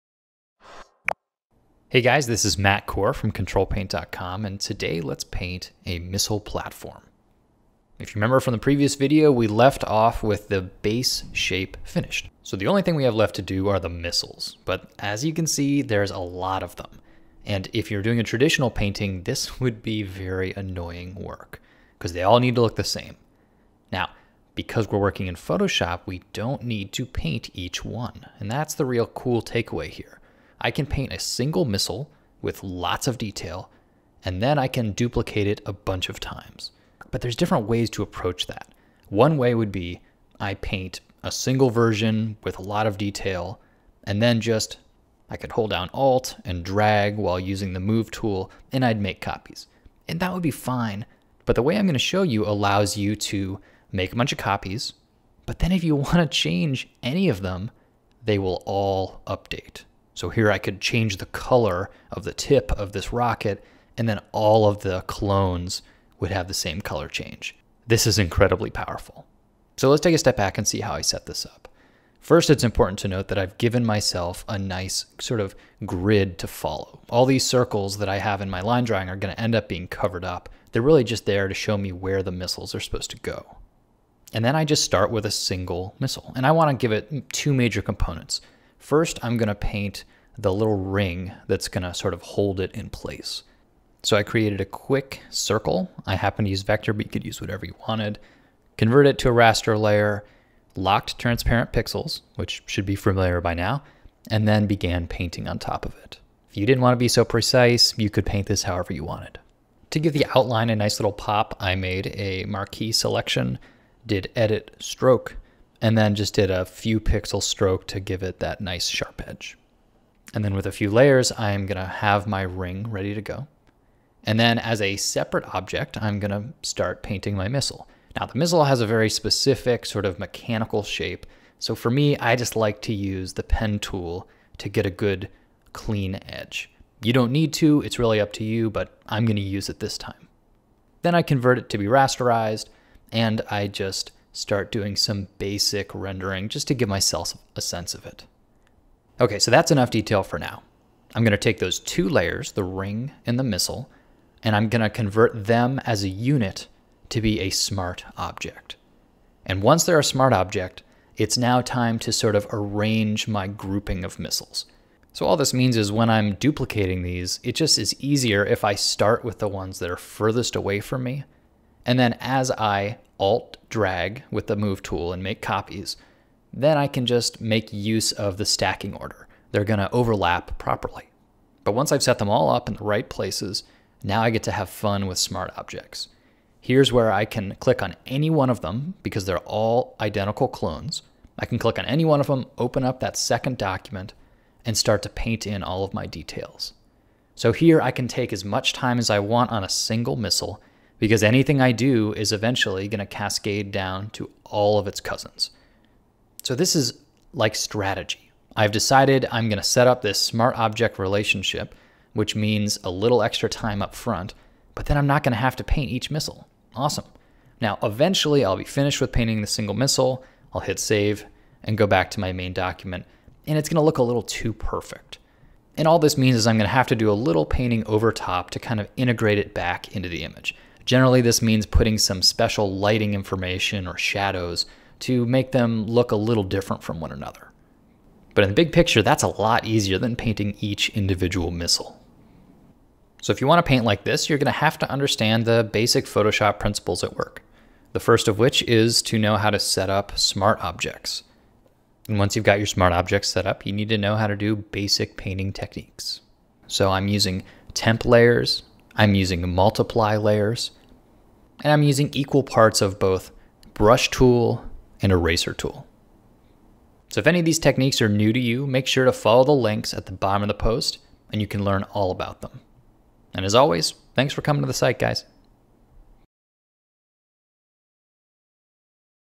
Let's Paint Missiles Pt.2 (CtrlPaint.com)
In the first half of this demonstration I painted the major masses of the missile platform.  Working in this manner, from simple to complex, is a common story on Ctrl+Paint.  There's no point in adding the detailed missiles if the underlying form isn't correct first.  But at this point in the demonstration, I'm comfortable with the foundation and it's time to add the detailed missiles.
The technique central to painting these missiles is called "smart objects",  and is a bit unexpected for those with traditional art roots.